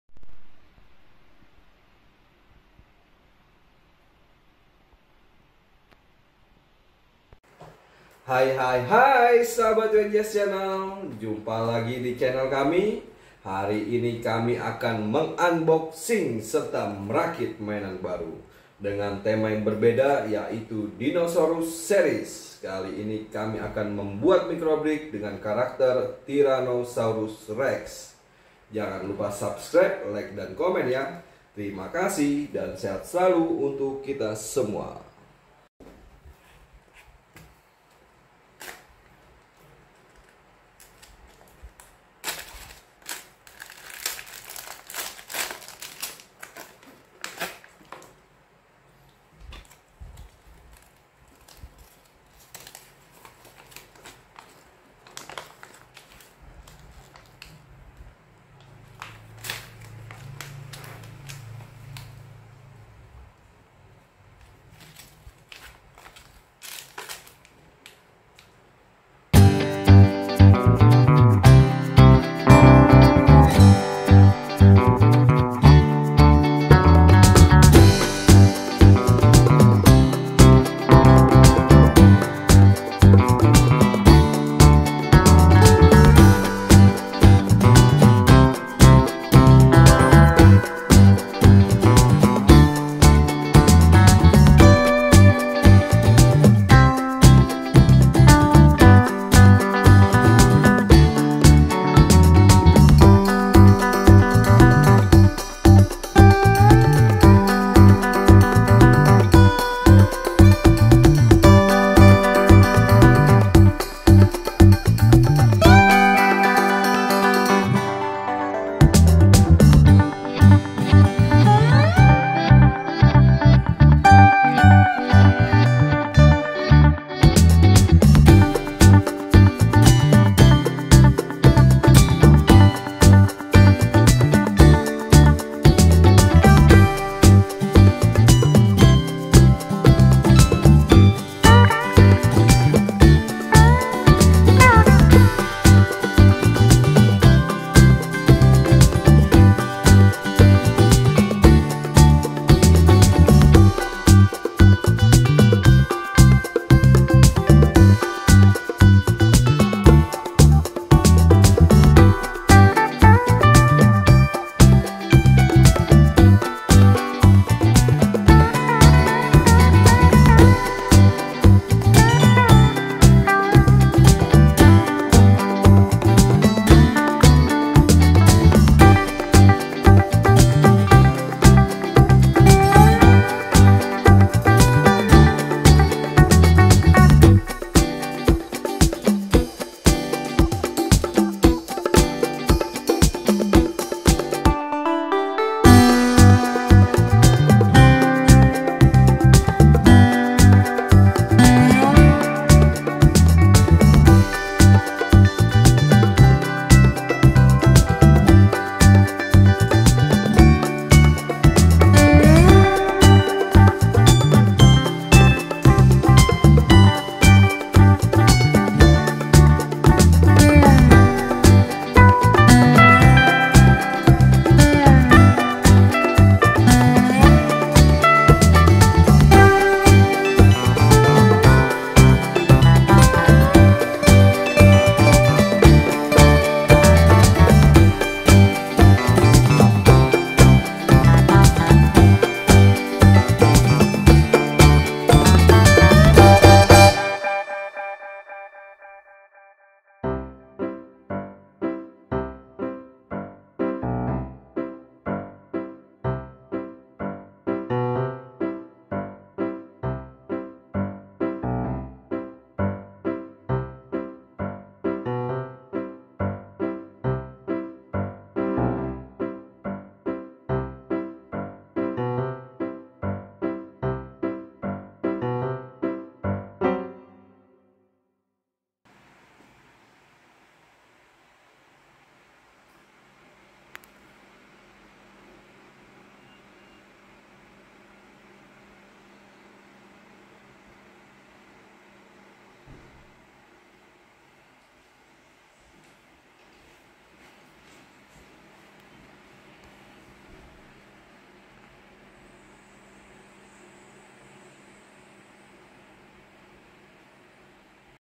Unboxing & Speed Build Toy Tyrannosaurus Rex / Mainan Micro Brick Seri Dinosaur
Pembuatan Mini Micro Brick Trannosaurus Rex Merek PETIT BLOCK No. JK - JM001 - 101.
Permainan ini disarankan untuk 12 tahun keatas ( 12+ ).

Terima kasih, sukses, sehat dan bahagia selalu untuk para penonton yang sudah menonton video kami.